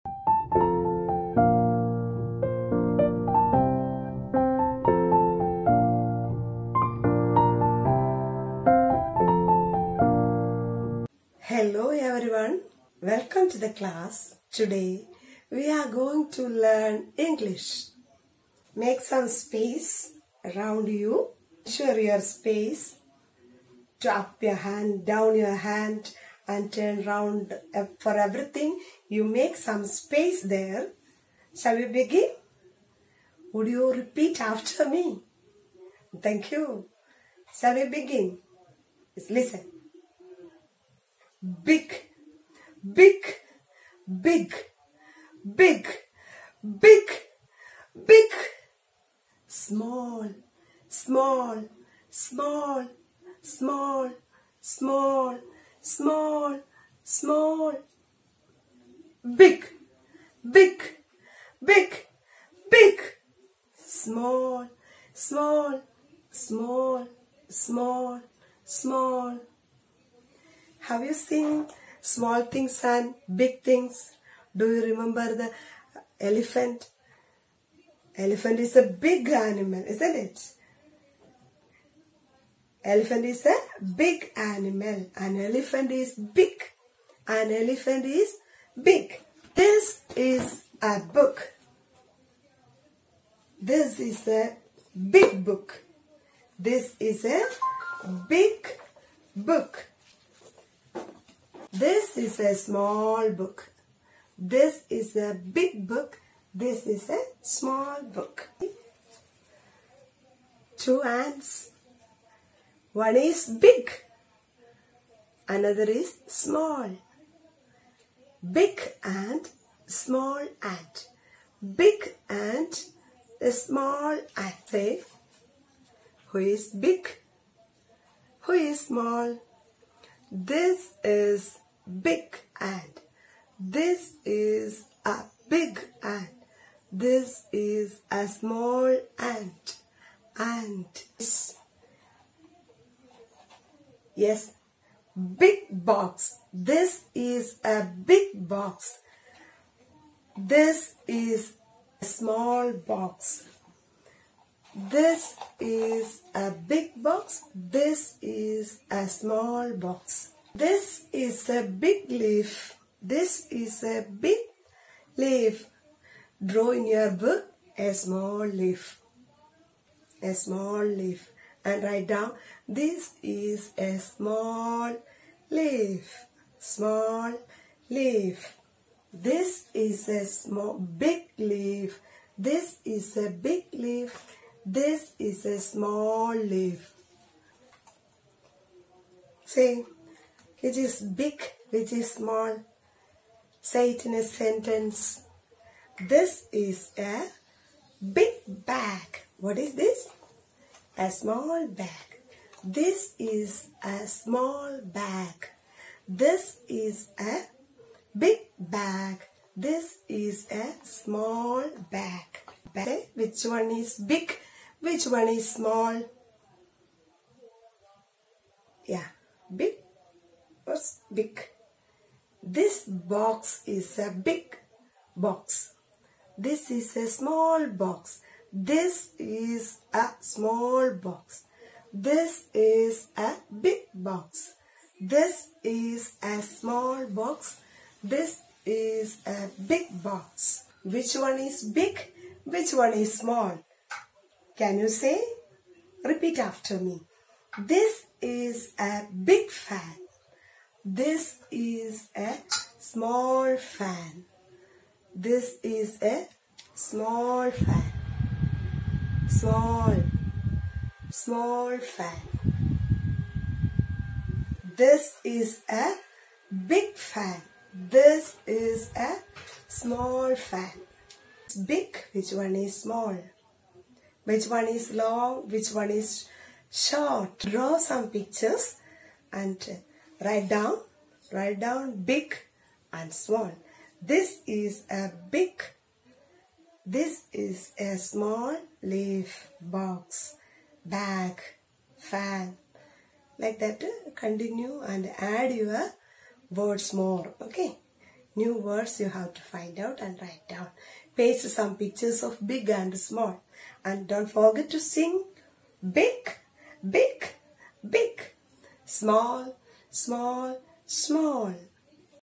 Beginner's english |LP |Let's learn english |learn big and small | Dr.Carmaly
This video is only for beginners in English.Video enables the learners to  practice different  language elements.
Video ensures maximum opportunities to enhance learning outcomes through life real situations.
This video imbibes clear  ideas of big and small.